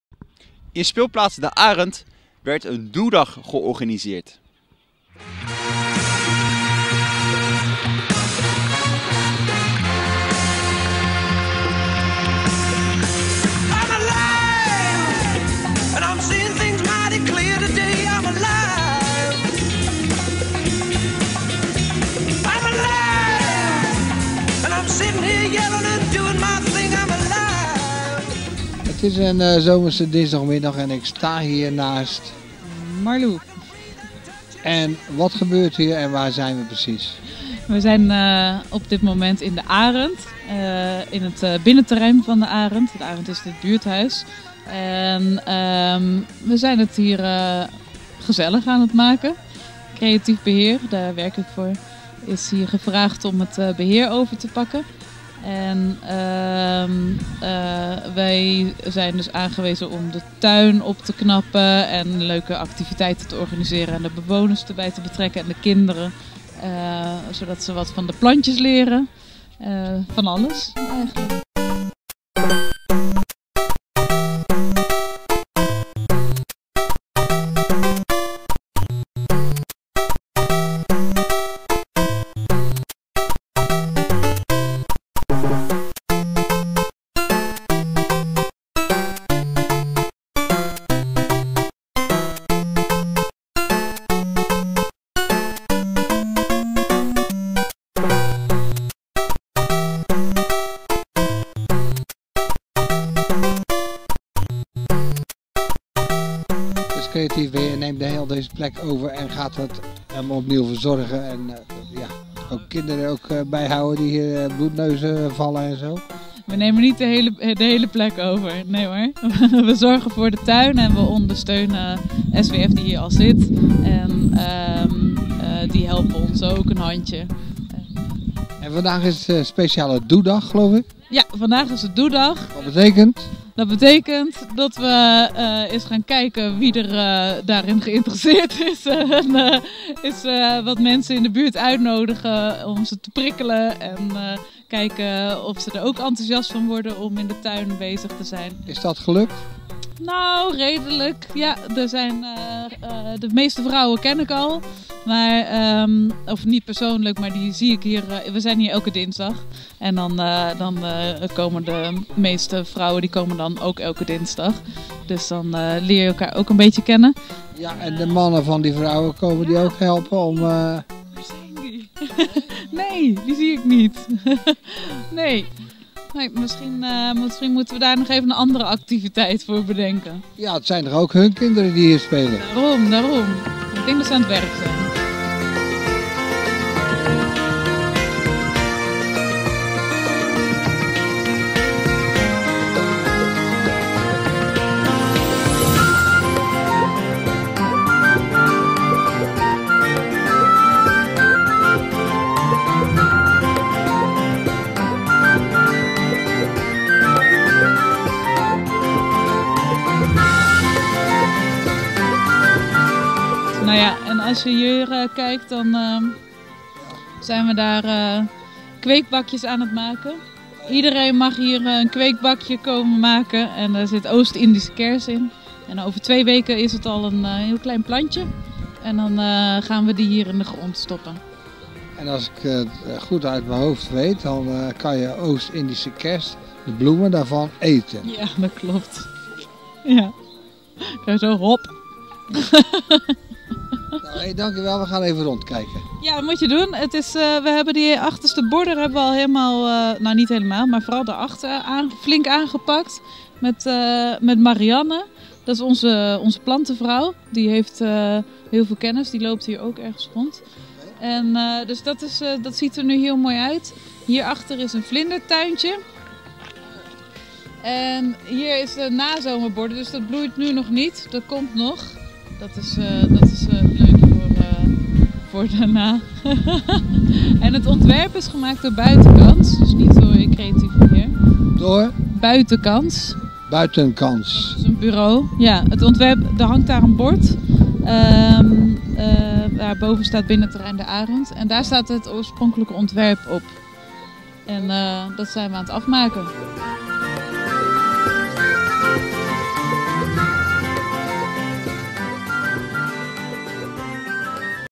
doedag.mp4
In buurthuis de Arend werd een doedag georganiseerd door Creatief Beheer.